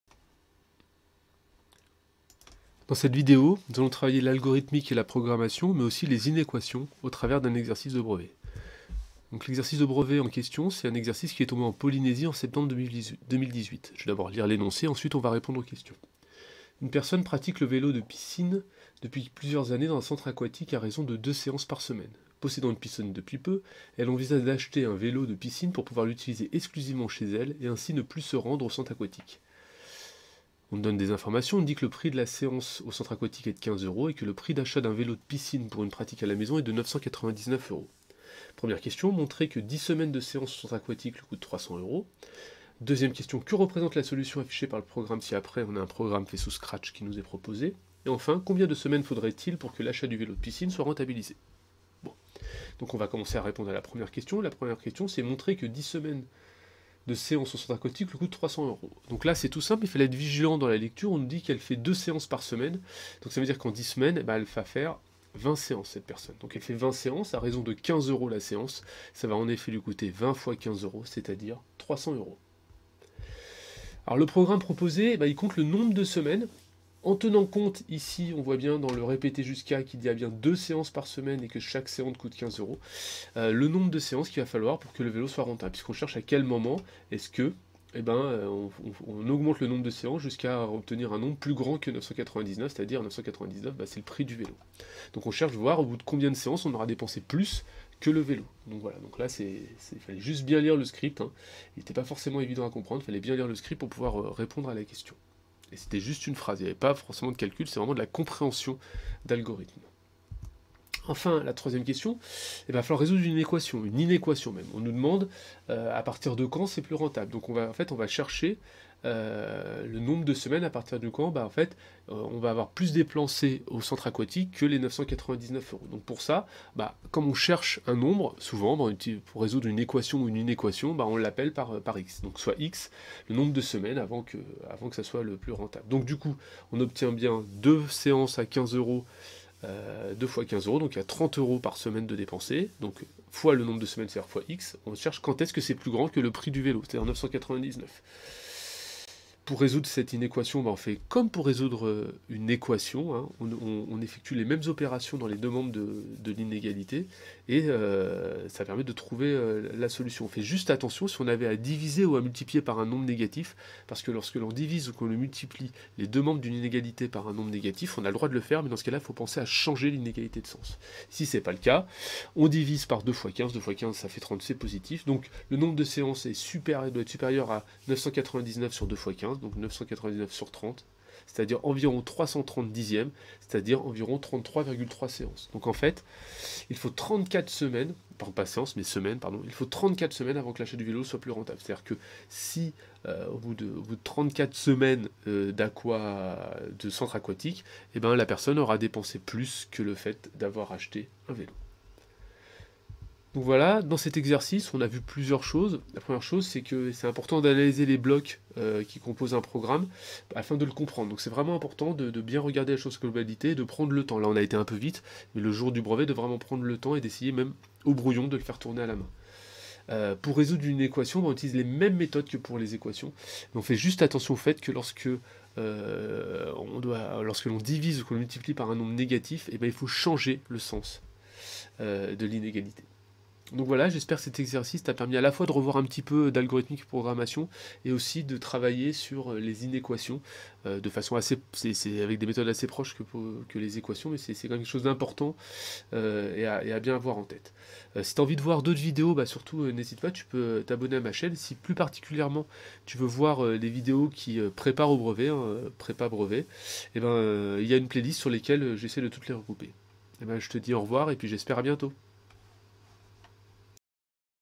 Prépa DNB : Algorithmique, programmation et inéquations
Exercice de brevet sur la compréhension d'un programme Scratch et la résolution d'une inéquation.